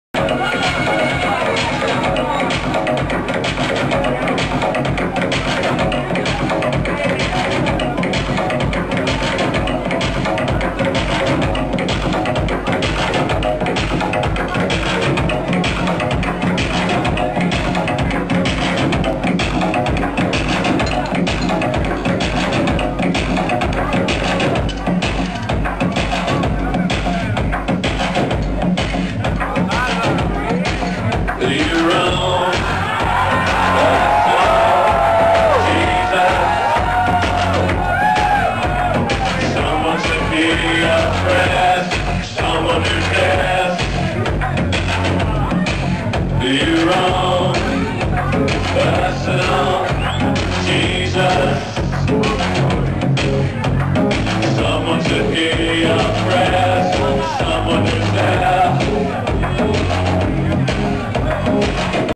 ronald k @ 809koro2, new age cafe (jul.06.2007)
me spinning @ new age cafe, July 6th, 2007. i was kind of drunk, and very happy, so excuse the dancing like a geek :p